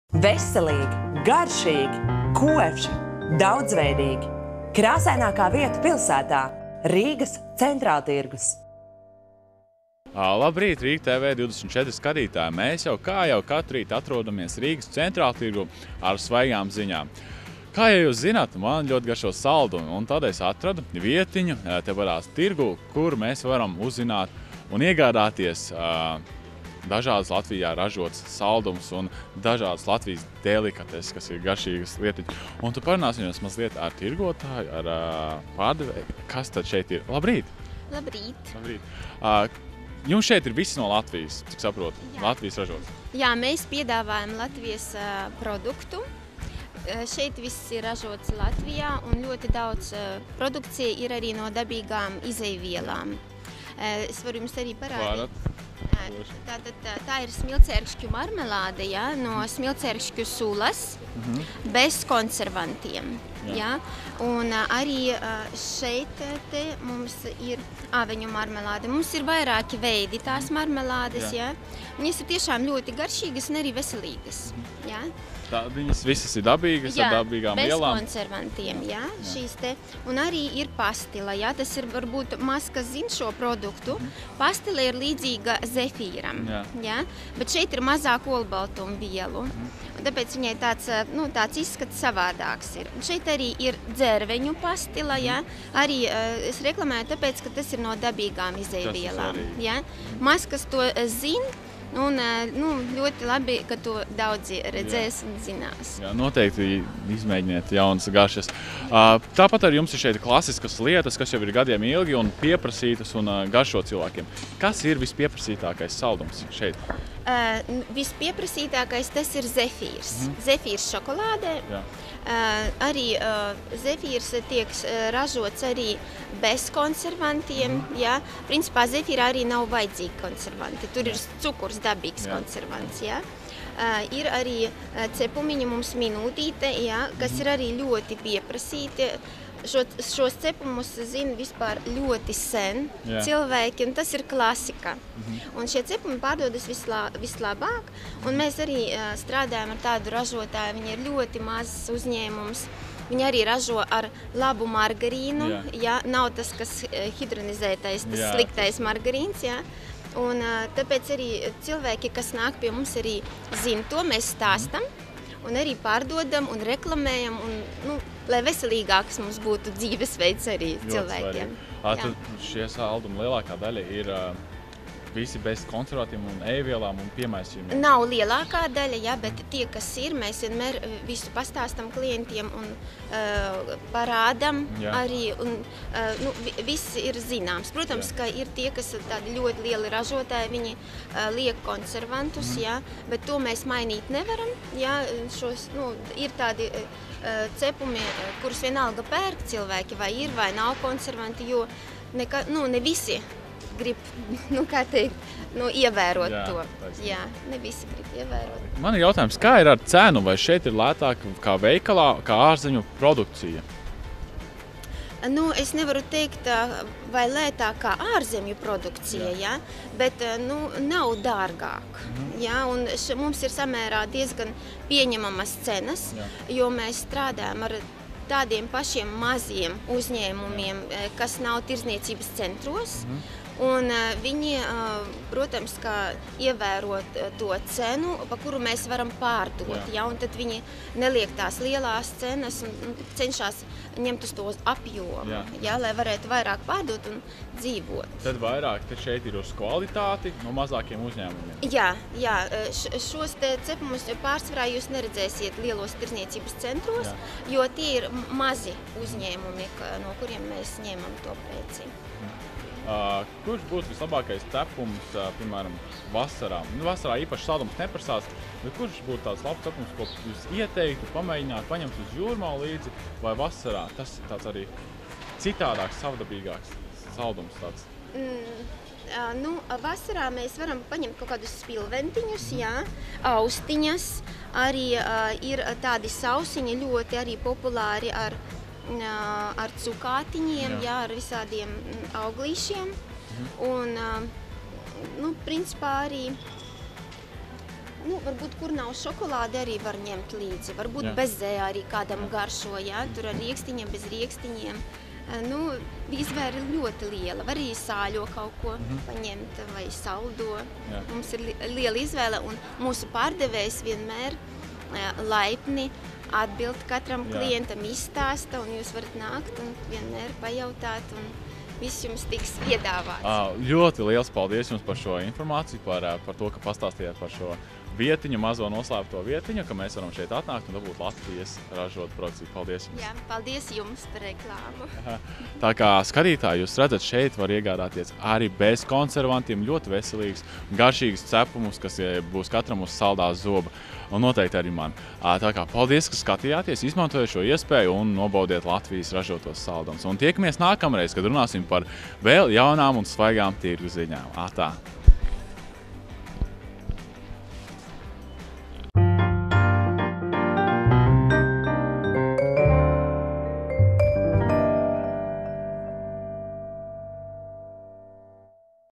Cepumu pasaulē
08.04. Šodien ielūkosimies lielajā cepumu stendā lai izvēlētos pašus gardākos, saldākos un siltām rokām lolotos cepumiņus!